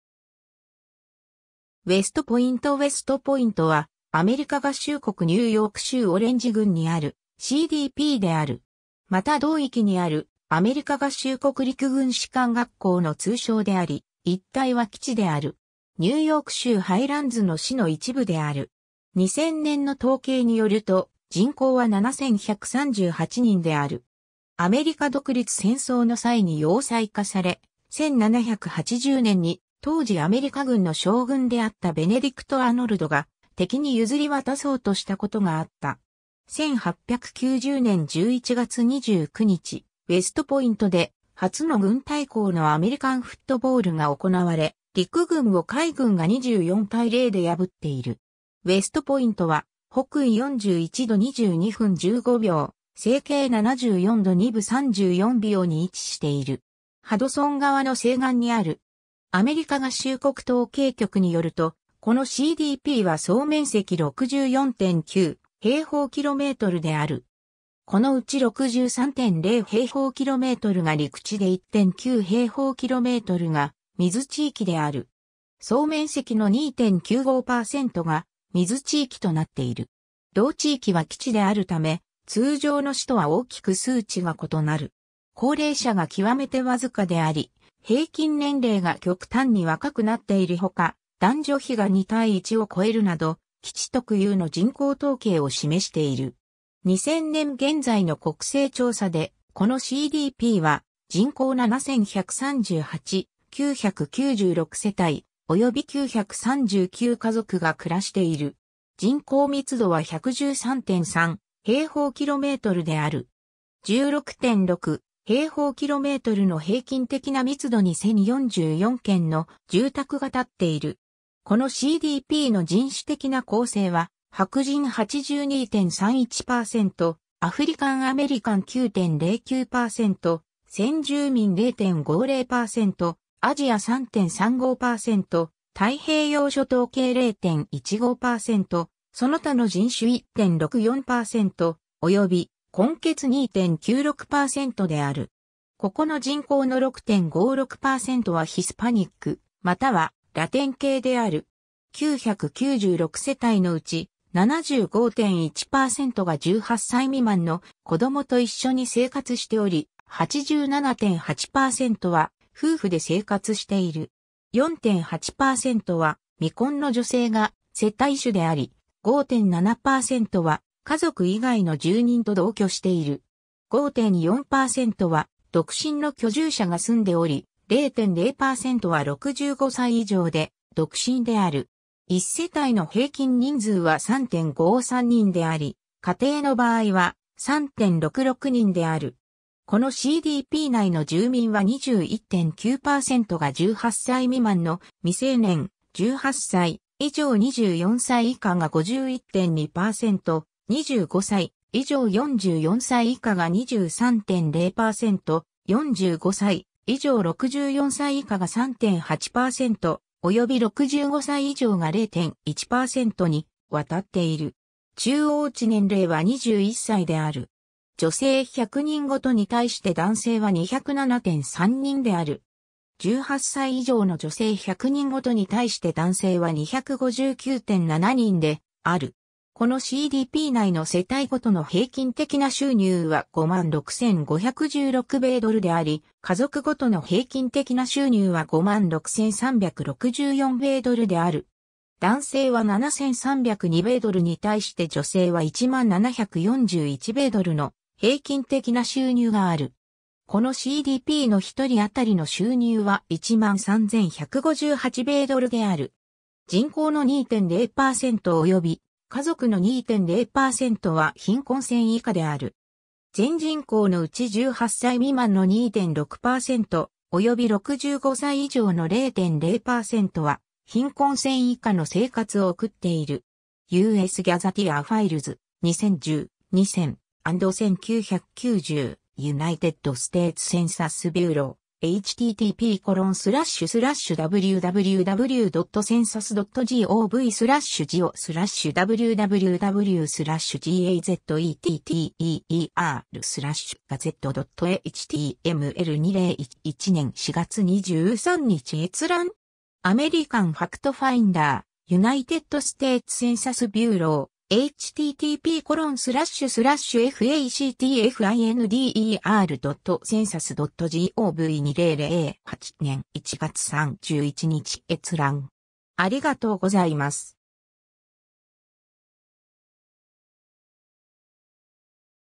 ウェストポイント (ニューヨーク州)
オレンジ郡_(ニューヨーク州)
ウェストポイント ウェストポイント (West Point) は、アメリカ合衆国ニューヨーク州オレンジ郡にあるCDPである。
また同域にあるアメリカ合衆国陸軍士官学校の通称であり、一帯は基地である。
ニューヨーク州ハイランズの市の一部である。
2000年の統計によると、人口は7,138人である。
 アメリカ独立戦争の際に要塞化され、1780年に当時アメリカ軍の将軍であったベネディクト・アーノルドが敵に譲り渡そうとしたことがあった。
 1890年11月29日、ウェストポイントで初の軍対抗のアメリカンフットボールが行われ、陸軍を海軍が24対0で破っている。
 ウェストポイントは、北緯41度22分15秒、西経74度2分34秒 (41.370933, -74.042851)に位置している。
ハドソン川の西岸にある。
 アメリカ合衆国統計局によると、この CDP は総面積64.9 km2 (25.1 mi2) である。
このうち63.0 km2 (24.3 mi2) が陸地で1.9 km2 (0.7 mi2) が水地域である。
 同地域は基地であるため、通常の市とは大きく数値が異なる。
高齢者が極めてわずかであり、平均年齢が極端に若くなっている他、男女比が2対1を越えるなど、基地特有の人口統計を示している。
 2000年現在の国勢調査で、この CDP は人口7,138、996世帯、及び939家族が暮らしている。
人口密度は113.3/km2 (293.4/mi2) である。
16.6/km2 (42.9/mi2) の平均的な密度に1,044軒の住宅が建っている。
この CDP の人種的な構成は白人82.31%、アフリカン・アメリカン9.09%、先住民0.50%、アジア3.35%、太平洋諸島系0.15%、その他の人種1.64%、及び混血2.96%である。
ここの人口の6.56%はヒスパニックまたはラテン系である。
 996世帯のうち、75.1%が18歳未満の子供と一緒に生活しており、87.8%は夫婦で生活している。
4.8%は未婚の女性が世帯主であり、5.7%は家族以外の住人と同居している。
5.4%は独身の居住者が住んでおり、0.0%は65歳以上で独身である。
1世帯の平均人数は3.53人であり、家庭の場合は、3.66人である。
 この CDP 内の住民は21.9%が18歳未満の未成年、18歳以上24歳以下が51.2%、25歳以上44歳以下が23.0%、45歳以上64歳以下が3.8%、及び65歳以上が0.1%にわたっている。
18歳以上の女性100人ごとに対して男性は259.7人である。
 この CDP 内の世帯ごとの平均的な収入は56,516米ドルであり、家族ごとの平均的な収入は56,364米ドルである。
この CDP の一人当たりの収入 (per capita income) は13,158米ドルである。
^ US Gazetteer files: 2010, 2000, and 1990, United States Census Bureau, (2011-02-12), 2011年4月23日閲覧。